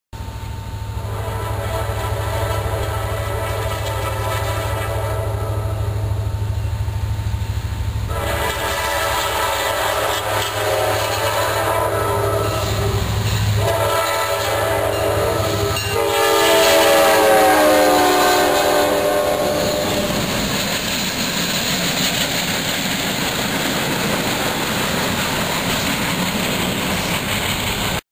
Norfolk Southern Locomotive ON FIRE !!!
January 23, 2008: NS 963, light engines, the 6th unit NS 6199 catches fire when the traction motor / armature fails and the roller bearing seizes. The 4th axle is burning away the traction motor, which causes a lot of sparks and flames to fly. Ties caught on fire, however smoldered out within a few minutes. Burnt shoes could be smelled over a half-hour period after the pass-by of the train. 963 consist of NS GP60 7125, NS Dash-9-40CW, NS Dash-8-40C, NS Dash-9-40C, NS RPU-6 864 Slug mated to NS SD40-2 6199.  Obviously, I called this in and the train stopped short of CP Prescott after passing over the 79.9 detector announcing 'no defects' - 963 was moving within 45 minutes. Taken in Richland. Enjoy the fireworks!